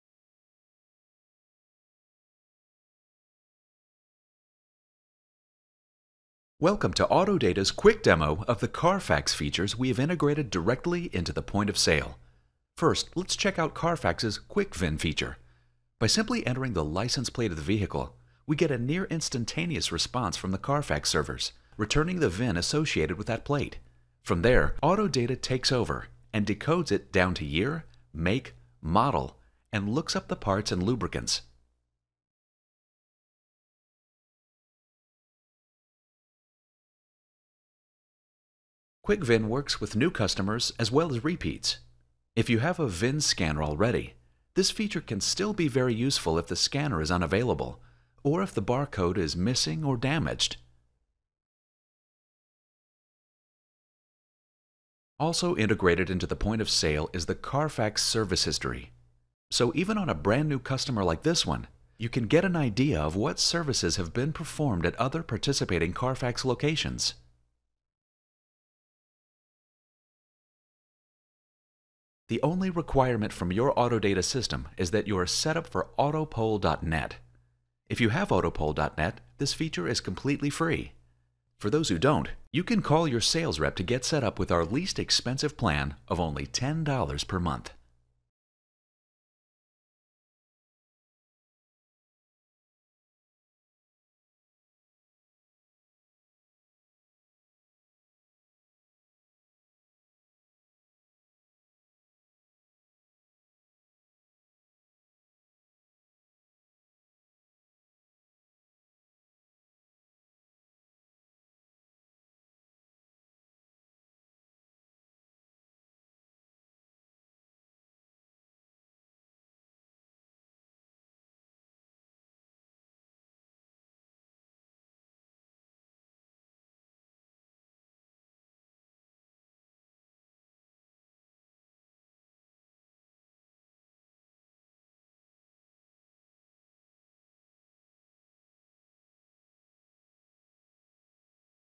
Quick CarFax Demo
A quick demonstration of Auto Data's Carfax integration.  QuikVIN and Carfax Service History are highlighted.